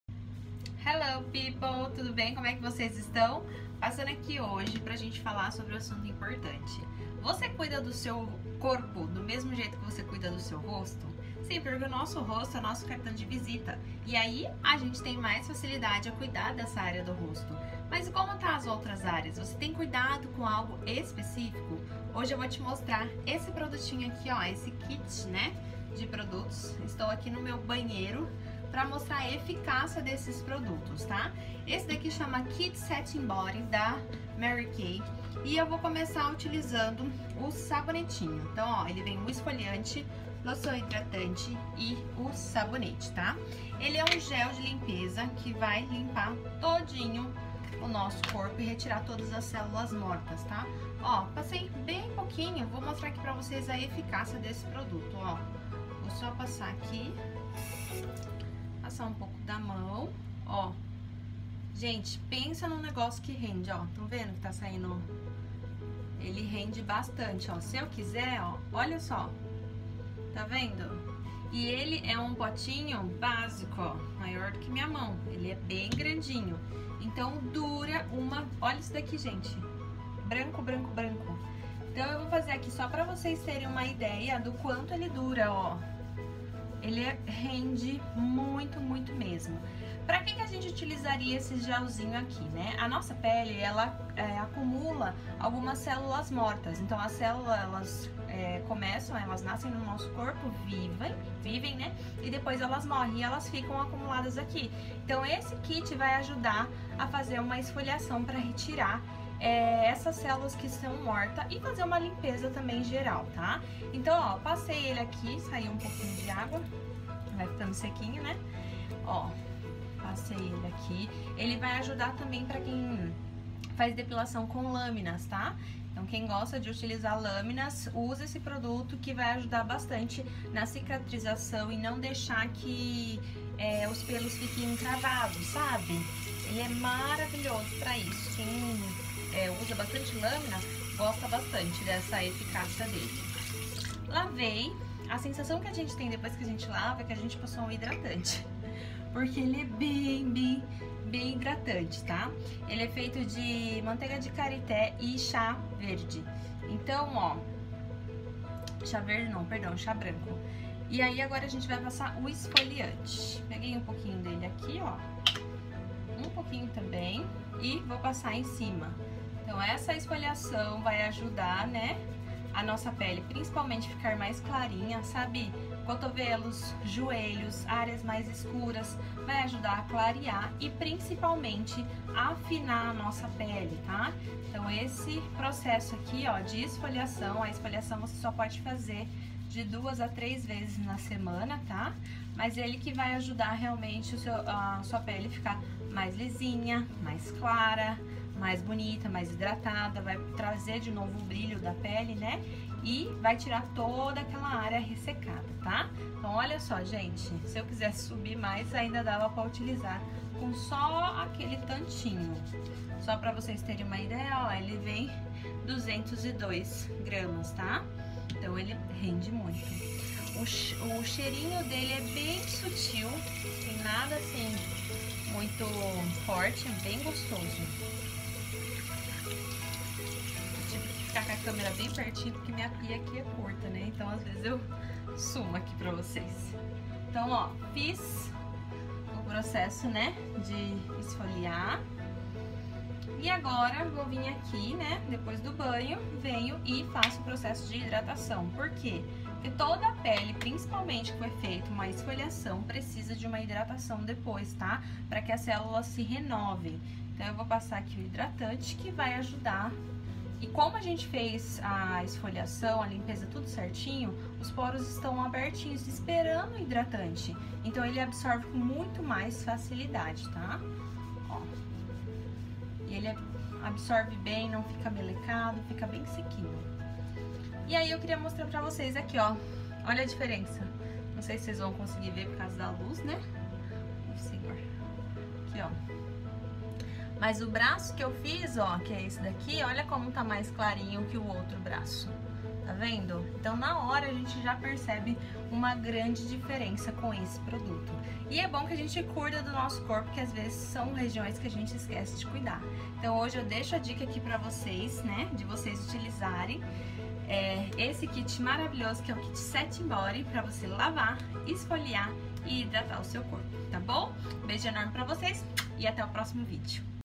Kit Satin Body Karité Chá Branco e Cítrus
Kit maravilhoso para cuidar do seu corpo. Composto por Manteiga de Karité Cítrus e Chá Branco esse kit promete acabar com áreas ressecadas, afinar sua pele, super hidratar e até clarear manchinhas. Quer saber mais?